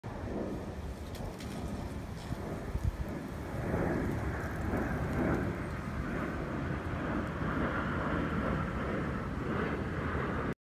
Tortoise Tries to Catch Ladybug || ViralHog
Occurred on October 24, 2019 / Clifton Park, New York, USA

"Scuter, a Sulcata tortoise, chasing a ladybug. The ladybug escaped."